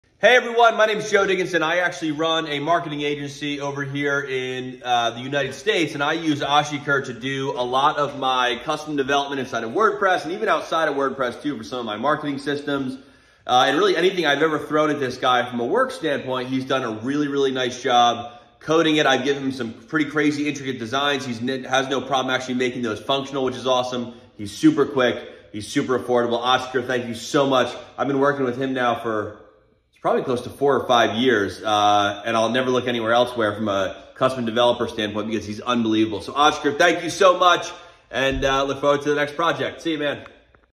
Joe Dickinson Testimonial About Ashik's Web Design & Development Expertise!
Check out what Joe Dickinson, a long-time collaborator, has to say about working with me for over 4 years! Dive into Joe's experience and see how my skills in web design and development helped bring his projects to life. Don't forget to explore more of my work